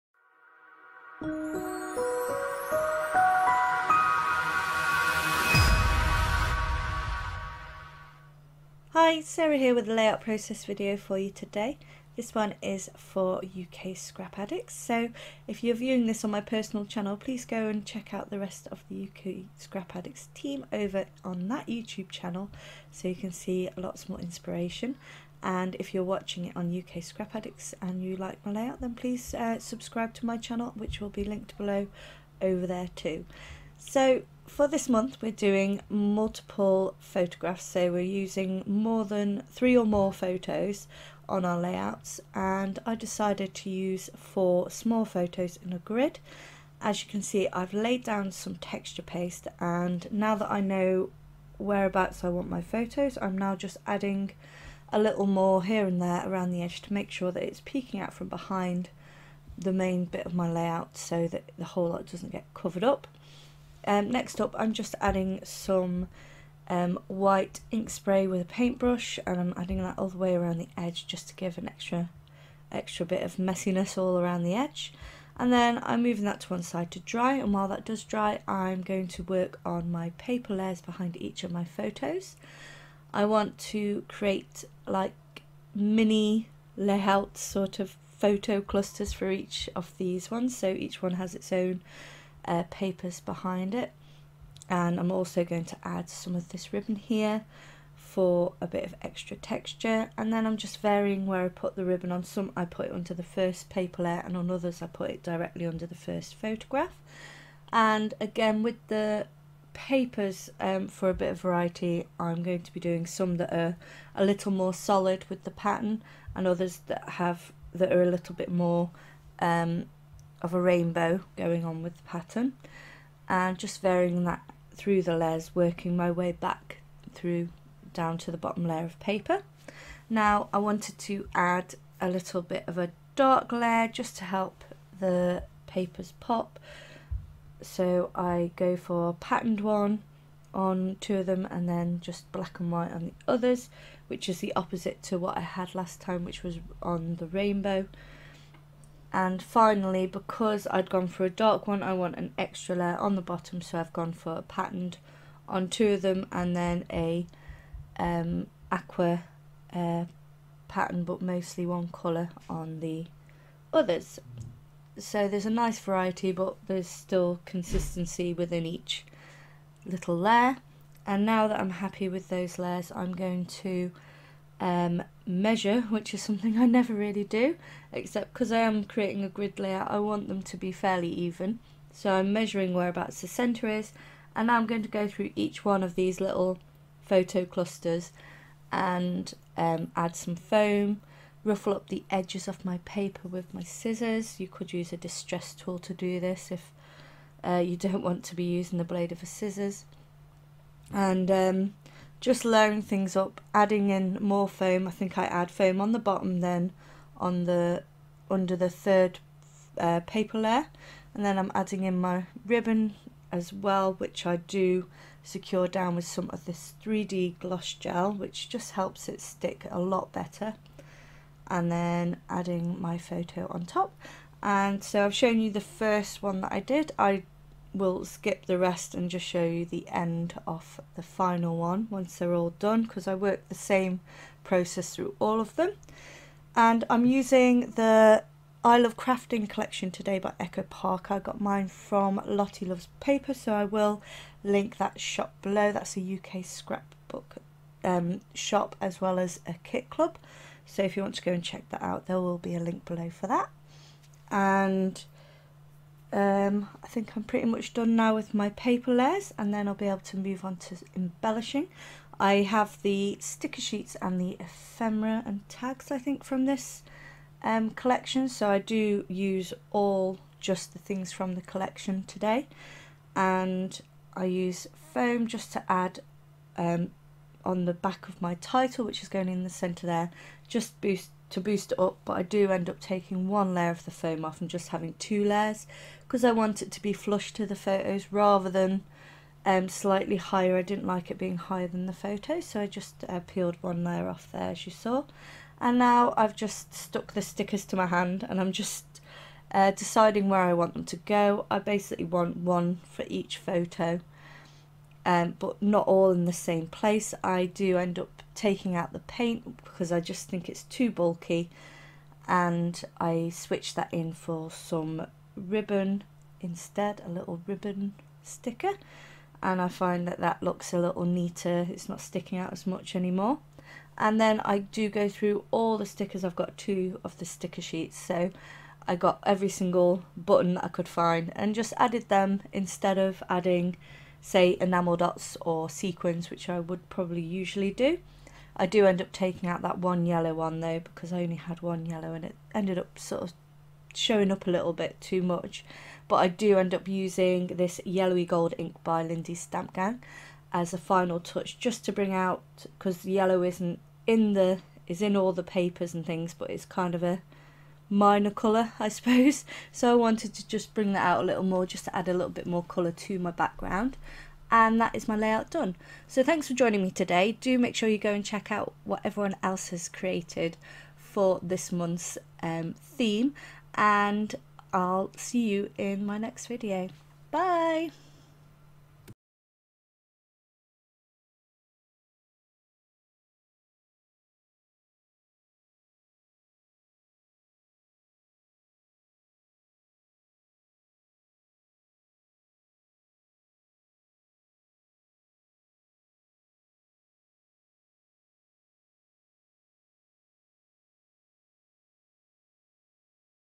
I'm Crafting | Sarah Costain | UK Scrap Addicts
Here's my layout for this months theme of multiple photos.

My Links :)